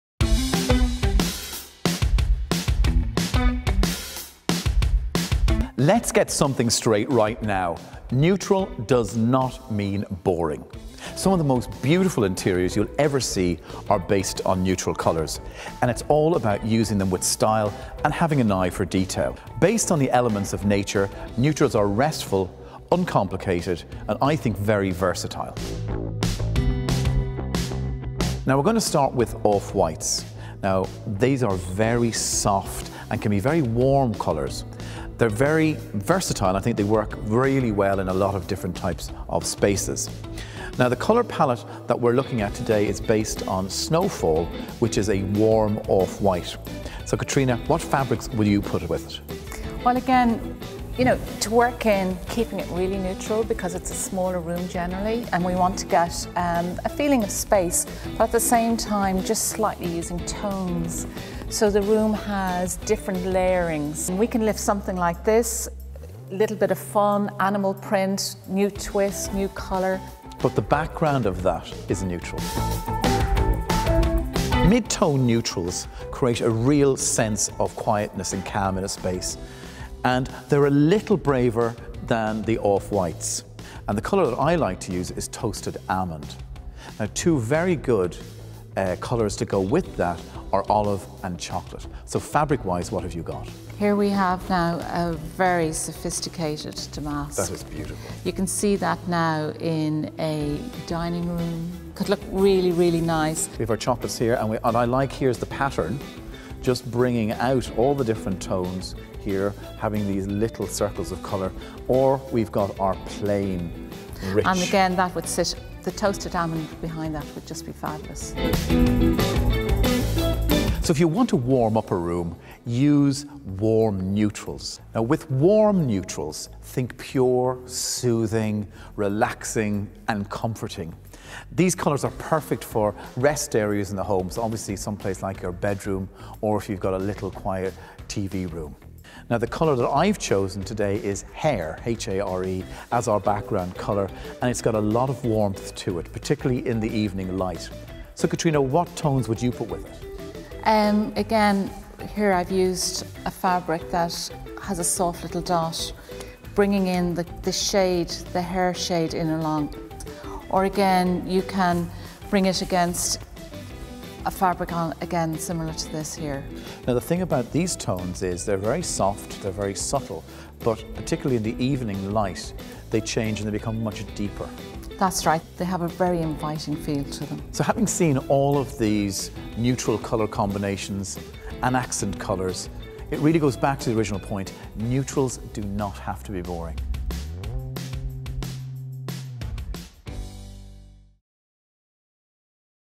Crown Paints Inspiration Series - Episode 3
Each month, top interior designer and Crown Paints Colour Consultant, Neville Knott will take us through the colours, trends and tips of the season!  In this episode, Neville, along with textiles expert Kathtrina Furlong, delves into world of neutral tones.